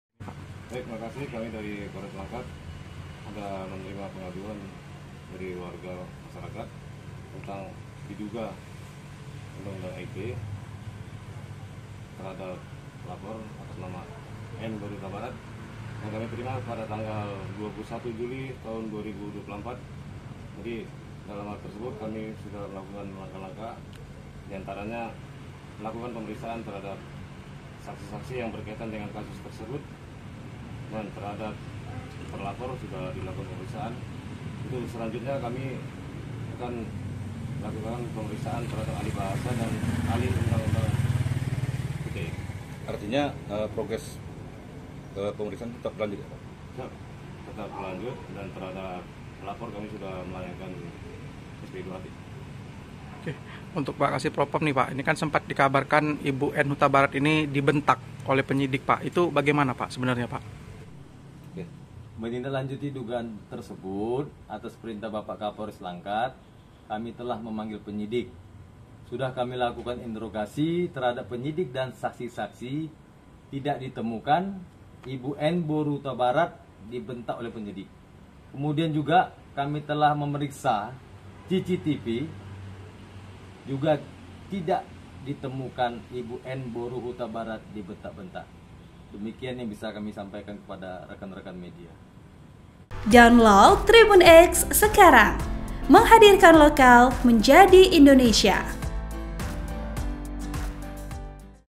PROPAM Polres Langkat Periksa Penyidik yang Diduga Bentak Personel Bhayangkari Korban Bully Rentenir
TRIBUN-MEDAN.COM - Kasus yang dialami seorang personel Bhayangkari bernama Nurmaslina Hutabarat (39) di Kabupaten Langkat, Sumatera Utara, masih hangat diperbincangkan di tengah-tengah masyarakat.

Pasalnya Nurmaslina diduga dibentak oleh penyidik Unit Tipidter Polres Langkat saat menanyai perkembangan kasusnya beberapa hari yang lalu.

"Menindaklanjuti dugaan tersebut, atas perintah bapak Kapolres Langkat, kami telah memanggil penyidik," ujar Kasi Propam Polres Langkat, Iptu F Hasibuan, Rabu (2/10/2024).

Lanjut Hasibuan, penyidik yang diduga membentak personel Bhayangkari itu sudah diinterogasi.

"Sudah kami lakukan interogasi terhadap penyidik dan saksi-saksi, tidak ditemukan Ibu N Hutabarat dibentak oleh penyidik," ujar Hasibuan.

"Kemudian kami juga telah memeriksa CCTV, dan tidak juga ditemukan Ibu N Hutabarat dibentak-bentak," sambungnya.

Sedangkan itu Kasi Humas Polres Langkat, AKP Rajendra Kusuma membenarkan jika pihaknya ada menerima pengaduan masyarakat (Dumas) diduga undang-undang ITE atasnama pelapor Nurmaslina Hutabarat.

"Laporan itu kita terima pada tanggal 21 Juli 2024. Dan saat ini kami sudah melakukan pemeriksaan saksi-saksi, terlapor juga sudah diperiksa," ujar Rajendra.

Selanjutnya, penyidik Unit Tipidter Polres Langkat juga akan periksa ahli bahasa, dan saksi undang-undang ITE.

"Intinya proses pemeriksaan tetap berlanjut, dan terhadap pelapor sudah kami kirim SP2HP," ujar Rajendra.

Dikabarkan sebelumnya, nasib malang dialami personel Bhayangkari di Kabupaten Langkat, Sumatera Utara, bernama Nurmaslina Hutabarat (39).

Pasalnya sudah empat tahun ia terus dirundung (Bully) oleh warga Desa Suka Damai Timur, Kecamatan Hinai, Kabupaten Langkat berinisial HA, RP, dan SUL yang sehari-hari bekerja sebagai rentenir.

Reporter: Muhammad Anil Rasyid
Editor Video: Abdan Syakuro